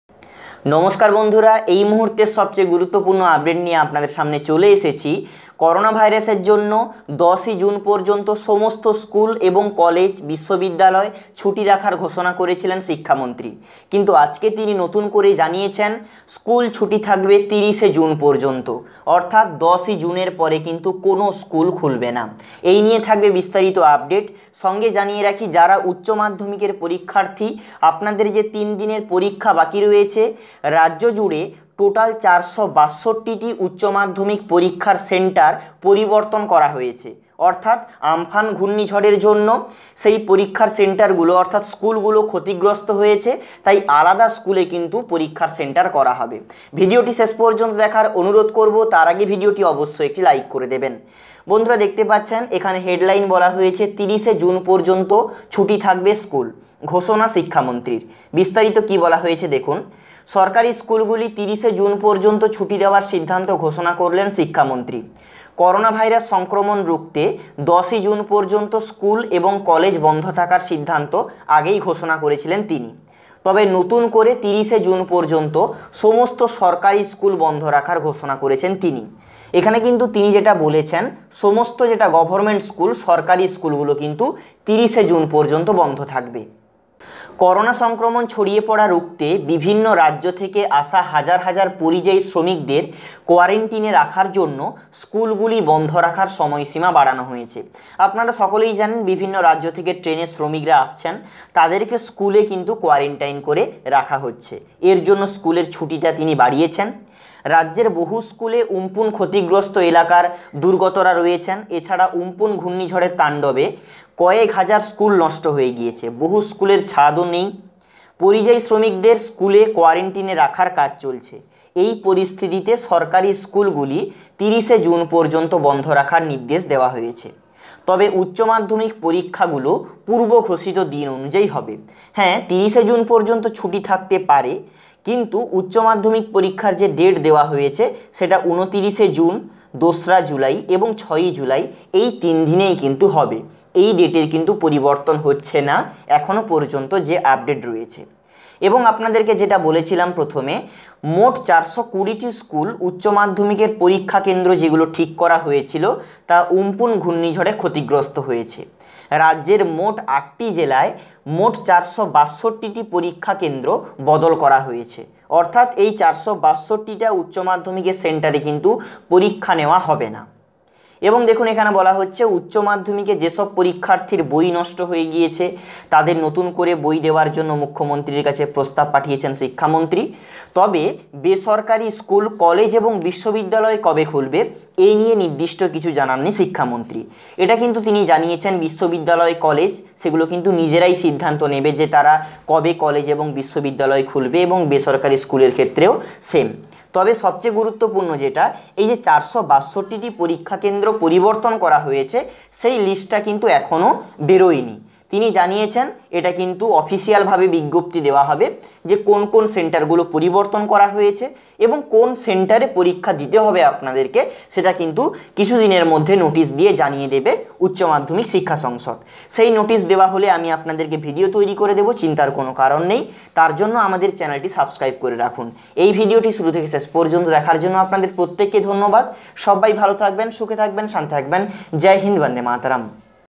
সরকারি স্কুলগুলি 30 জুন পর্যন্ত ছুটি থাকবে || 462 টি H.S.পরীক্ষার সেন্টার পরিবর্তন করা হলো!
🌏 Daily Current Affairs আপডেট পেতে
 আমাদের টেলিগ্রাম গ্রুপে যুক্ত হন-